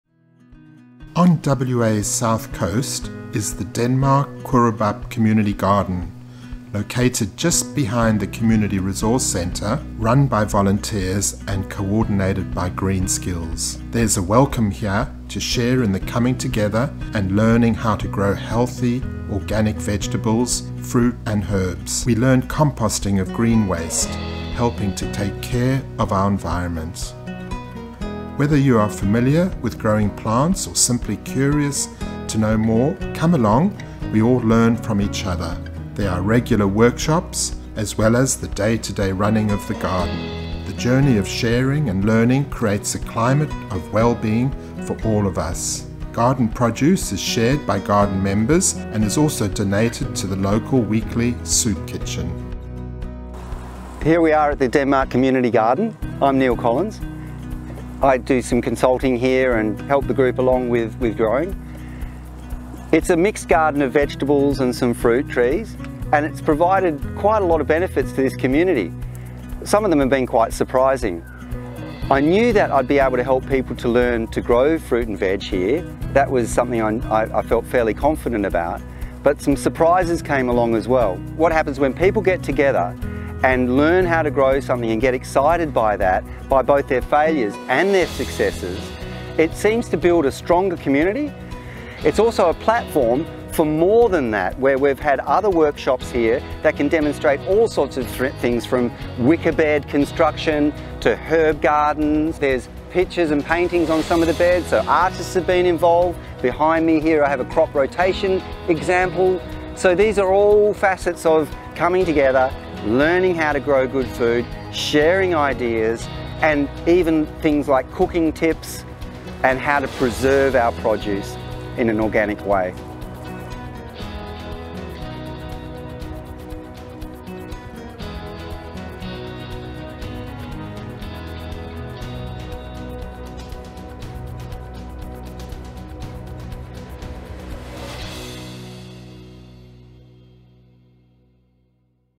Denmark Kwoorabup Community Garden
The Denmark Kwoorabup Community Garden is sited beside the Denmark Comunity Resource Centre ( CRC )in the south coast town of Denmark in Western Australia

People interested in participating can contact the Garden through Green SKills ( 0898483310) or the Denmark CRC